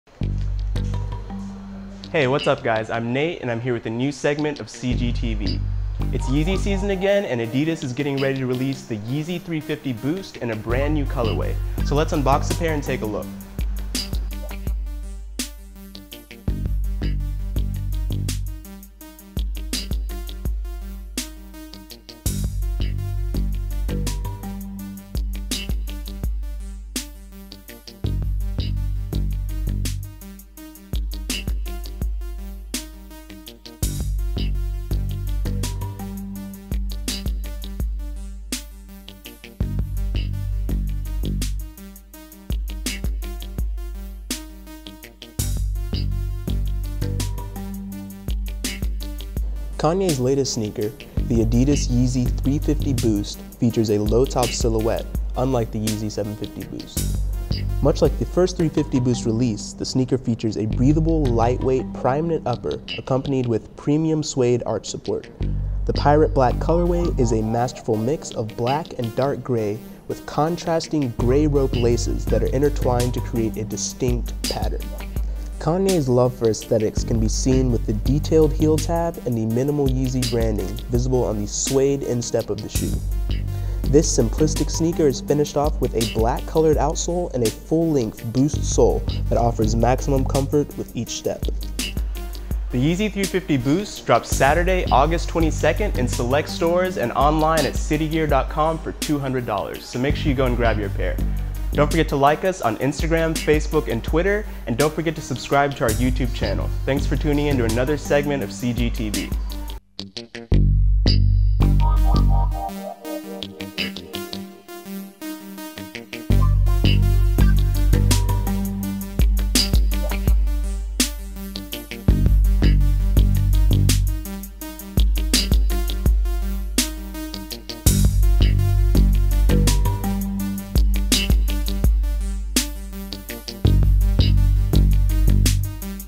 adidas Yeezy 350 Boost Black Shoe Review and On Feet Review
adidas Yeezy 350 Boost - SKU: AQ2659 - $200 - Launch: 8/22/15

Don't forget to check out